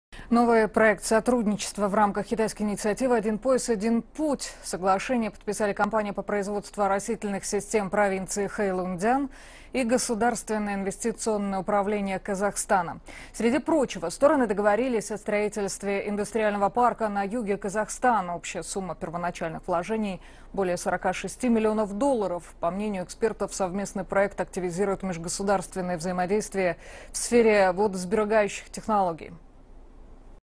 Провинция Хэйлунцзян инвестирует в Казахстан(один пояс, один путь))
Сотрудничество вдоль “Пояса и пути”

Телеканал CCTV-русскийЦентральноготелевиденияКитая.